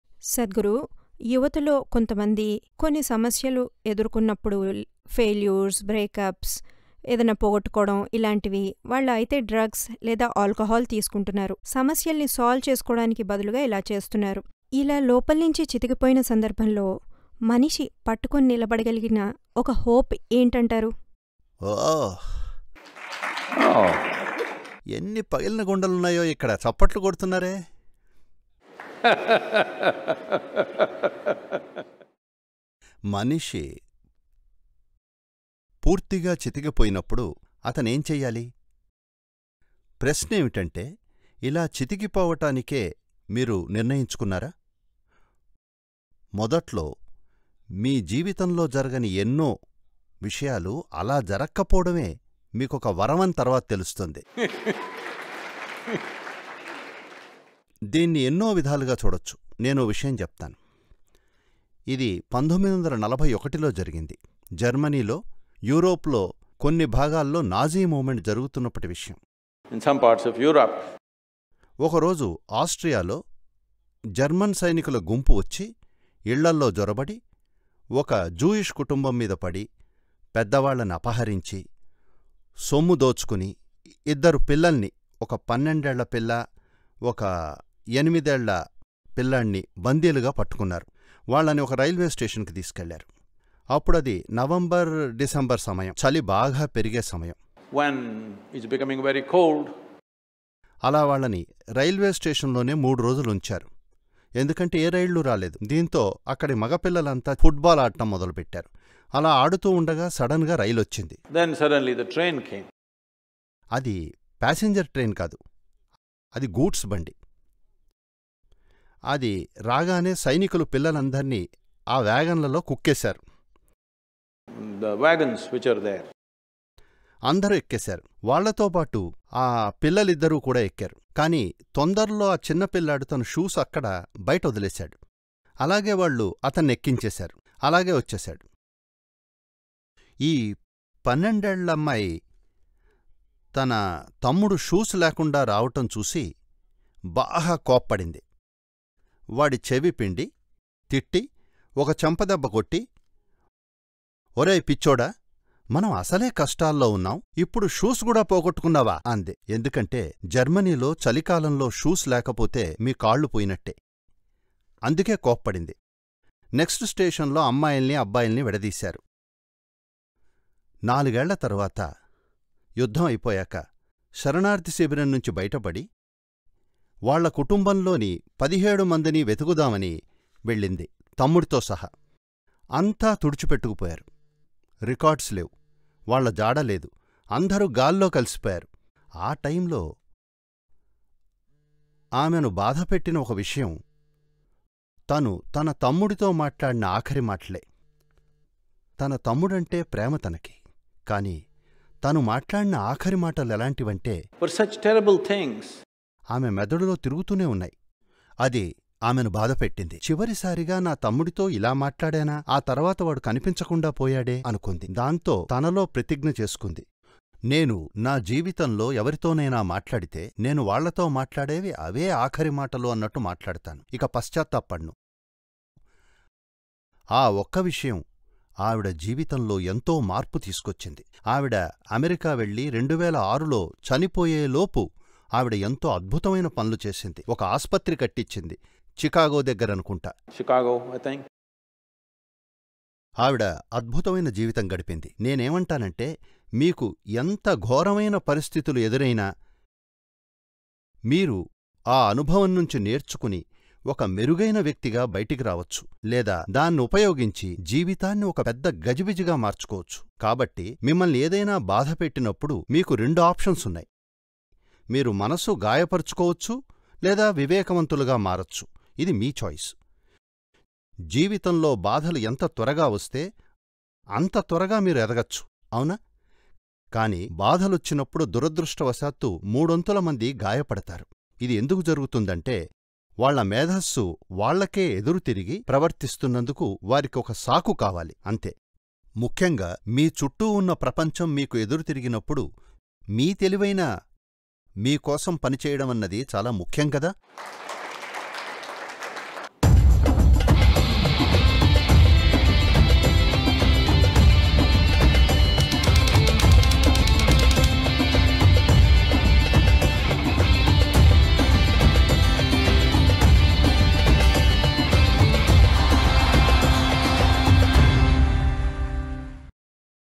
జీవితం భారంగా మారితే ఎలా నెగ్గుకురావాలి? Jeevitham Bharanga Marithe Yela Neggukuravali?
జీవితంలో క్లిష్టమైన పరిస్థితులు ఫెయిల్యూర్, బ్రేక్ అప్ లాంటివి మనల్ని అణిచివేస్తున్నప్పుడు మనం ఎలా బయటపడాలో సద్గురు ఒక నిజమైన కథ ద్వారా మనకు మార్గం చూపిస్తున్నారు.